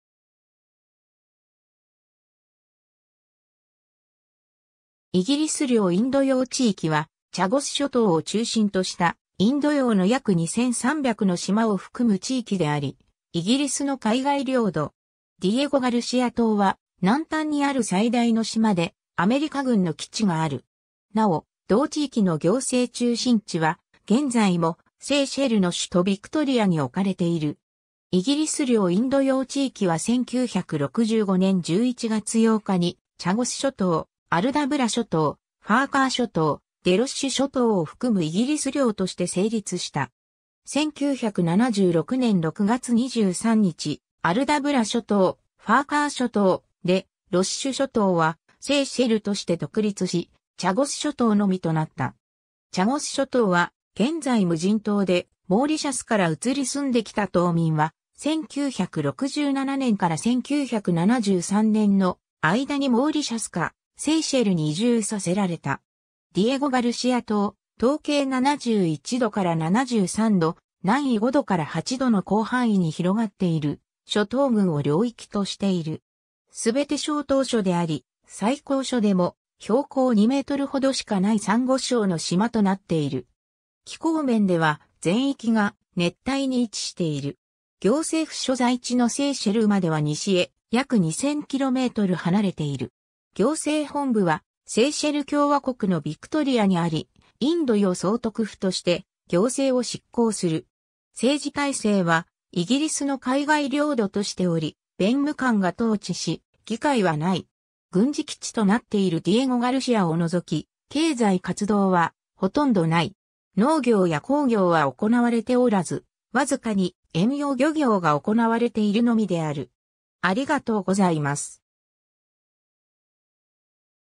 イギリス領インド洋地域
イギリス領インド洋地域（イギリスりょうインドようちいき、英語: British Indian Ocean Territory）は、チャゴス諸島を中心としたインド洋の約2300の島を含む地域であり、イギリスの海外領土。
ディエゴガルシア島は、南端にある最大の島で、アメリカ軍の基地がある。
なお、同地域の行政中心地は現在もセーシェルの首都ヴィクトリアにおかれている。
 イギリス領インド洋地域は1965年11月8日にチャゴス諸島、アルダブラ諸島、ファーカー諸島、デロッシュ（Des Roches）諸島を含むイギリス領として成立した。
1976年6月23日、アルダブラ諸島、ファーカー諸島、Des Roches諸島はセーシェルとして独立し、チャゴス諸島のみとなった。
チャゴス諸島は現在無人島で、モーリシャスから移り住んできた島民は、1967年から1973年の間にモーリシャスかセーシェルに移住させられた。
 ディエゴガルシア島 東経71度から73度、南緯5度から8度の広範囲に広がっている諸島群を領域としている。
全て小島嶼であり、最高所でも標高2mほどしかないサンゴ礁の島となっている。
気候面では全域が熱帯に位置している。
行政府所在地のセーシェルまでは西へ約2,000km離れている。
 行政本部はセイシェル共和国のビクトリアにあり、インド洋総督府として行政を執行する。
政治体制はイギリスの海外領土としており、弁務官が統治し、議会はない。
 軍事基地となっているディエゴガルシアを除き、経済活動はほとんどない。
農業や工業は行われておらず、わずかに遠洋漁業が行われているのみである。